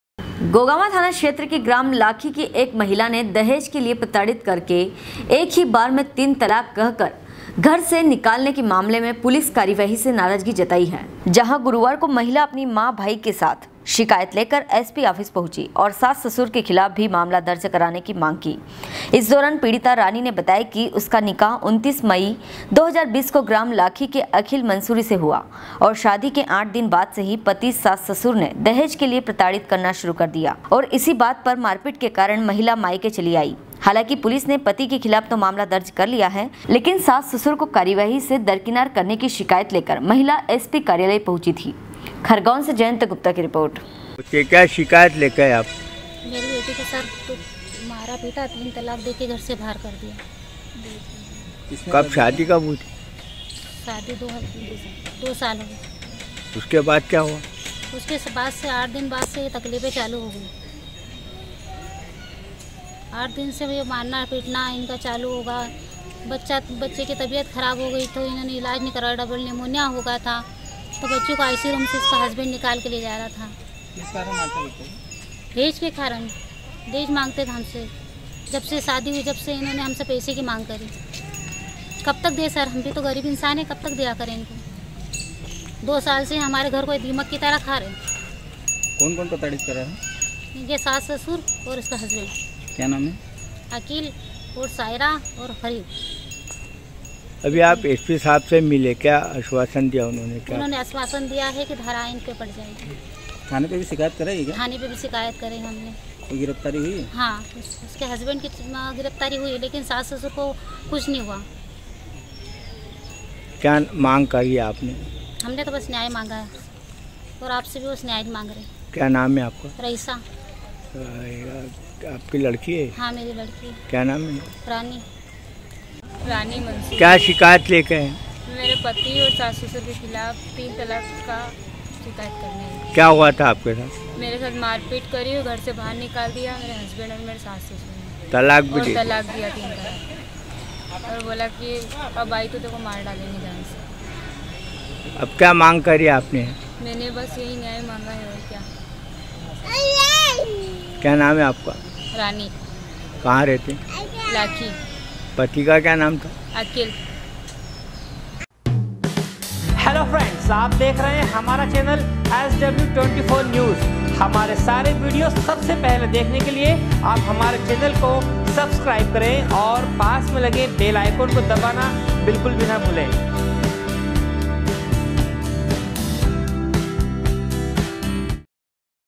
गोगावां थाना क्षेत्र के ग्राम लाखी की एक महिला ने दहेज के लिए प्रताड़ित करके एक ही बार में तीन तलाक
गोगावां थाना क्षेत्र के ग्राम लाखी की एक महिला ने दहेज के लिए प्रताड़ित करके एक ही बार में तीन तलाक कहकर घर से निकालने के मामले में पुलिस कार्रवाई से नाराजगी जताई है। खरगोन से जयन्ता गुप्ता की रिर्पोट।